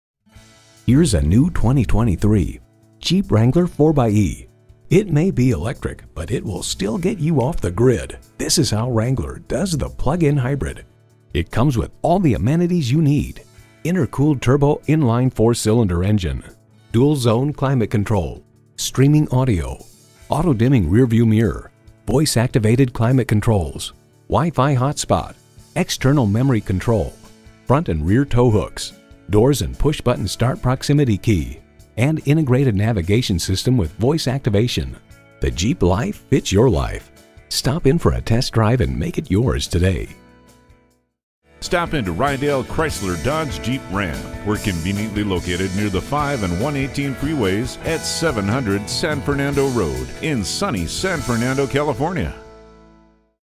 New 2023 Jeep Wrangler 4xe San Fernando CA Valencia, CA #650822
Year: 2023
Make: Jeep
Model: Wrangler 4xe
Trim: WRANGLER RUBICON 4xe
Engine: 2.0L I4 DOHC DI Turbo PHEV Engine
Transmission: 8-Speed Automatic 8P75PH PHEV Transmission
Color: Bright White
Interior: Black
Stock #: 650822
VIN: 1C4JJXR61PW650822

Address:
700 San Fernando Rd.